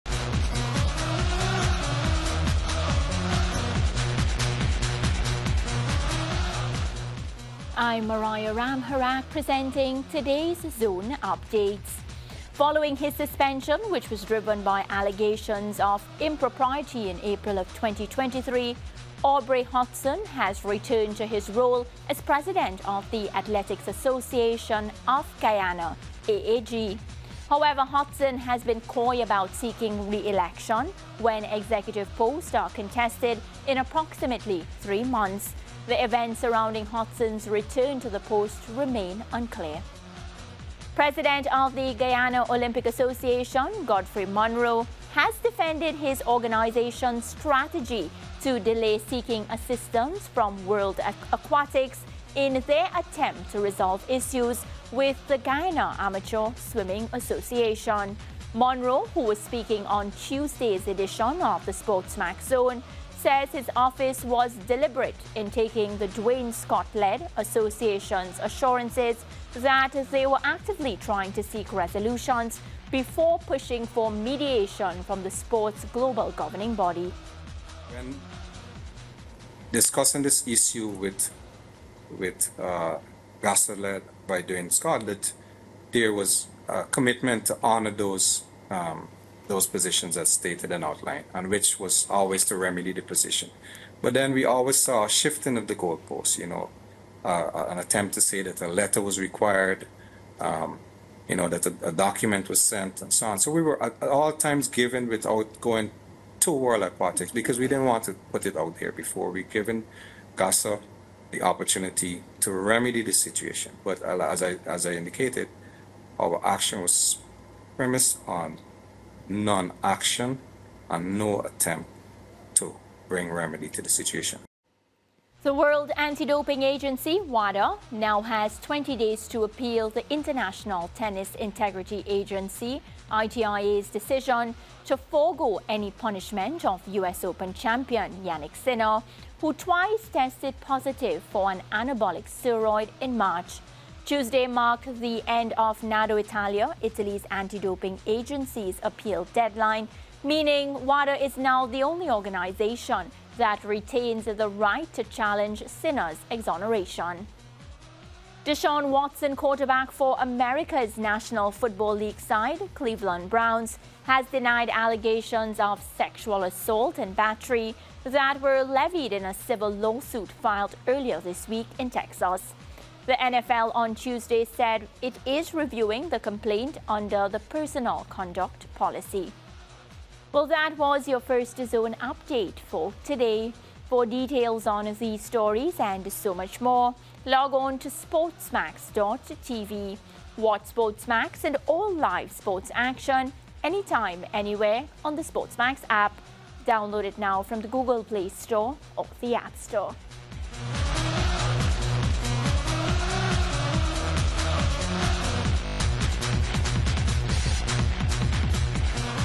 NSC & Guyana Olympics Association to formally engage World Aquatics | SportsMax Zone
National Sports Commission & Guyana Olympics Association to formally engage World Aquatics